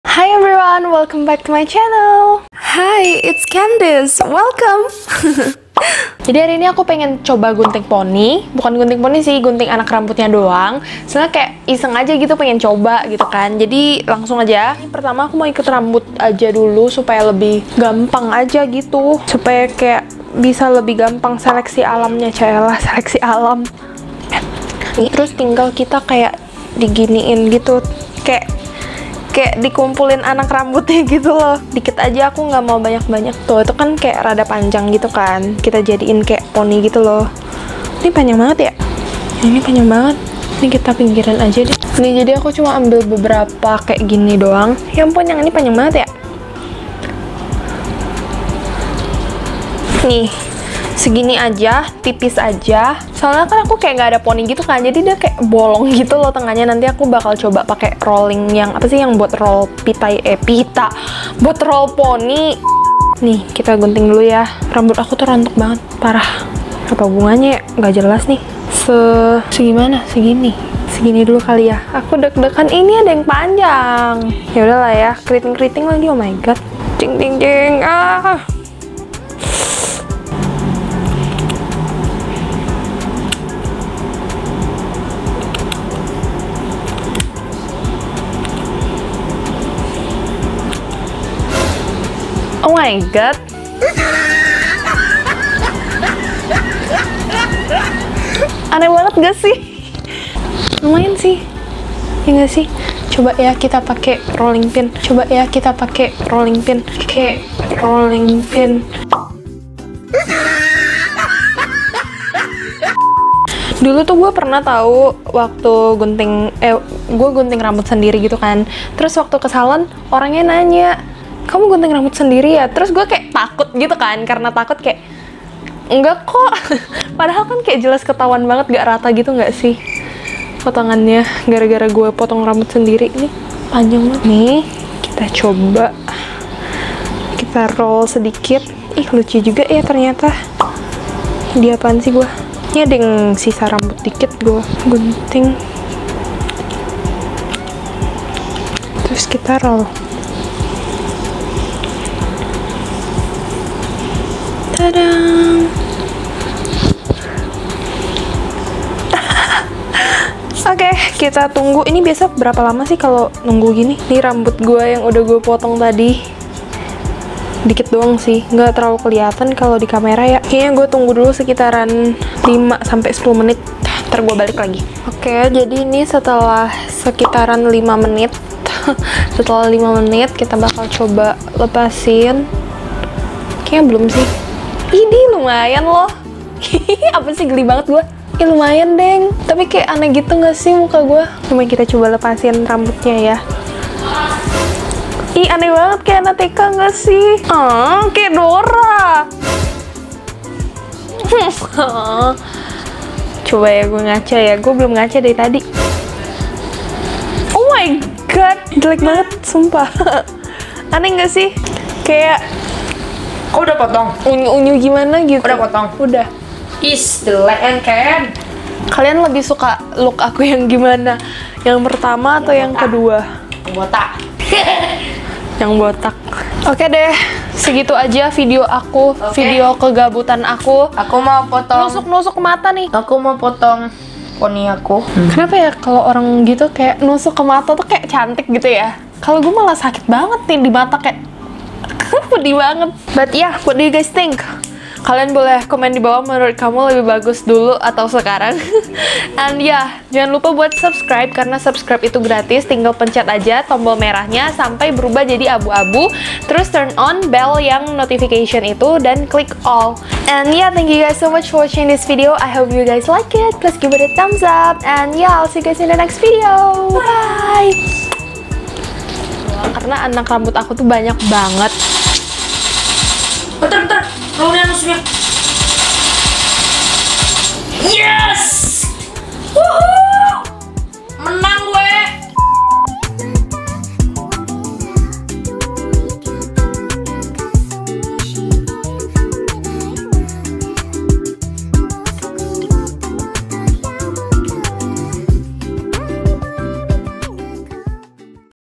LAH KOK MALAH JADI GINI.....| POTONG PONI SENDIRI EDISI DI RUMAH AJA
ini video kegabutanku mencoba hal hal baru hahaha kali ini aku coba potong anak rambut aku sendiri,iya tipis tipis aja karna aku tau kalo potongnya tebel mukaku tuh ga cocok kalo berponi hahaha

don't forget to smile cause you're special :)